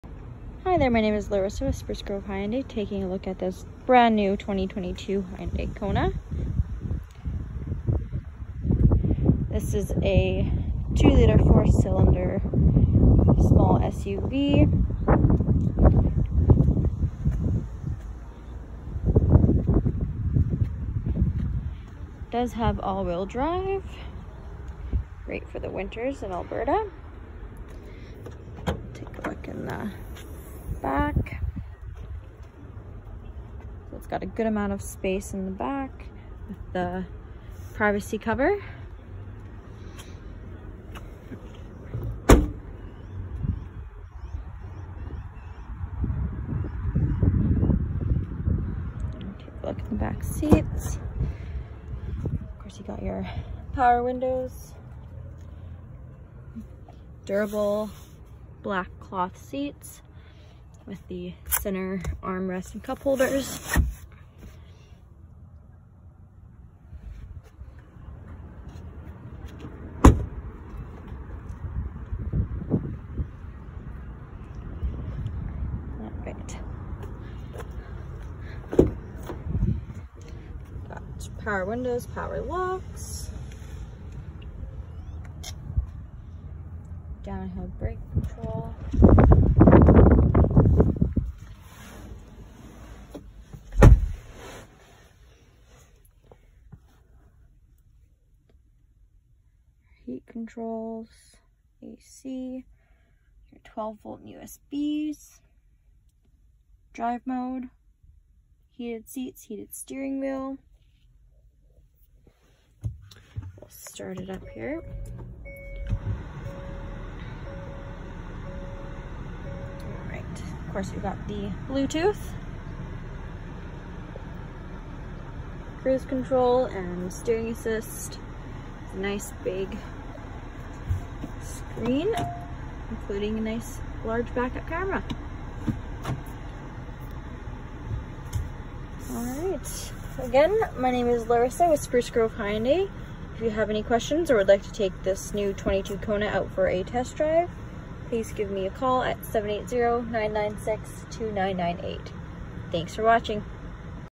Brand New 2022 Hyundai KONA Preferred!
Available at Grove Hyundai!
320 St. Matthews Ave Spruce Grove

HYUNDAI
2022 KONA
PREFERRED: AWD/SMARTSENSE/HEATED SEATS/APPLE CARPLAY/PROXY KEY